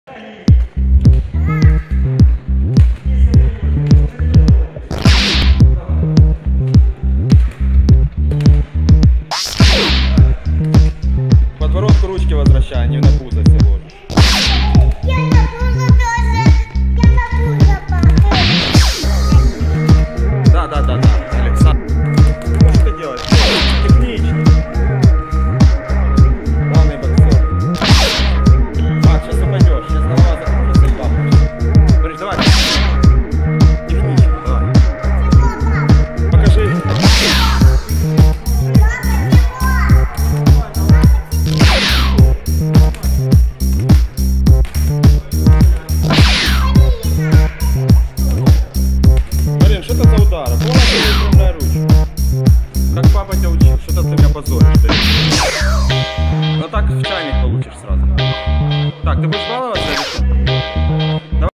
Kids Boxing Federation Cherkassy
- Федерация Бокса в Черкасской Области, маленькая девочка боксерша, боксирует грушу.
Отработка прямых ударов. Бокс, спорт, дети.
Маленькие дети бокс.